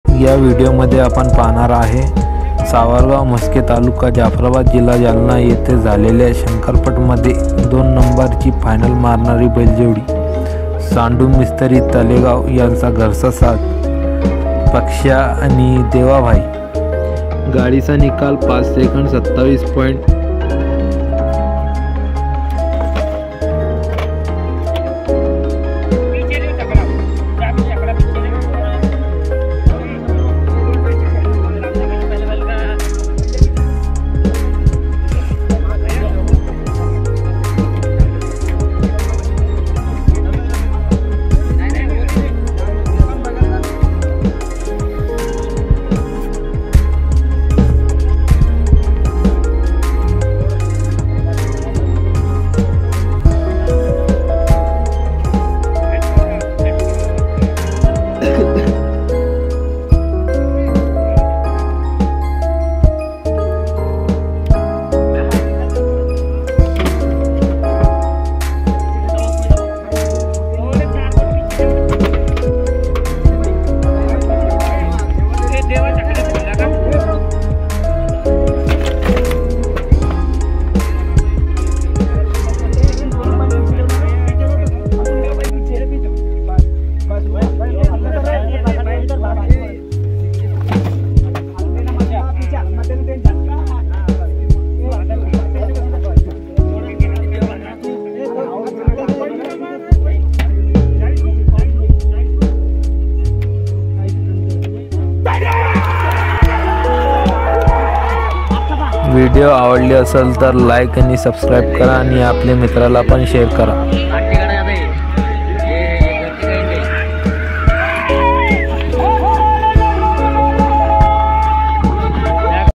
मैदानात धुमाकूल घालणारी बैलजोडी देवाभाई आणि पक्ष्या | खासदार केसरी देवाभाई शंकरपटात कसा धावतो बघ
सांडू मिस्तरी तळेगाव यांच्या पक्षाची बैलाची एक आठवण